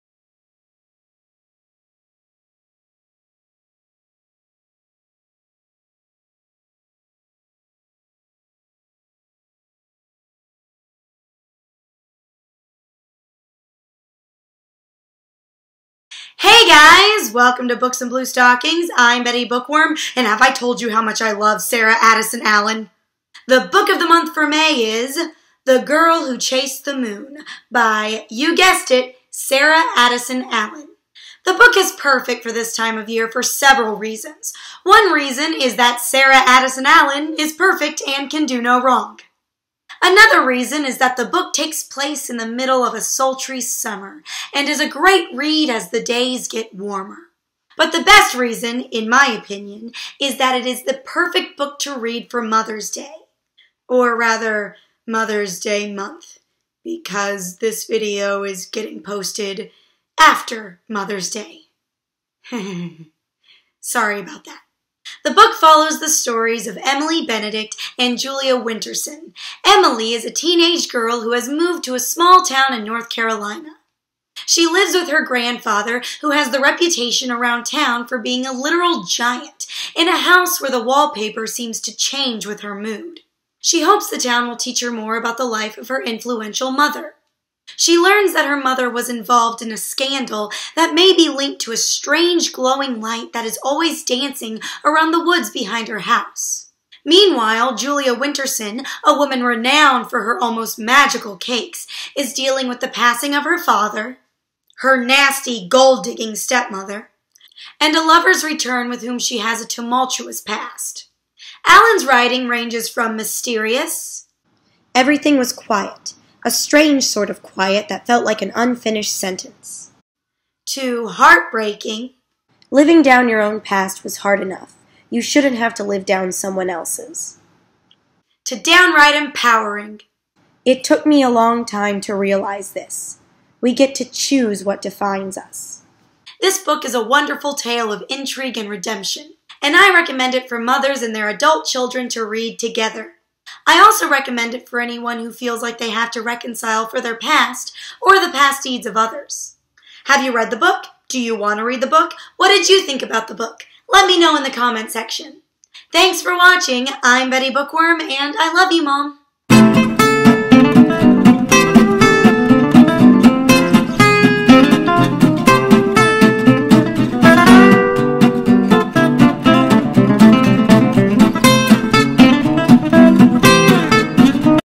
Books and BlueStockings- BOTM The Girl Who Chased The Moon
The Book of the Month for May is The Girl Who Chased the Moon by Sarah Addison Allen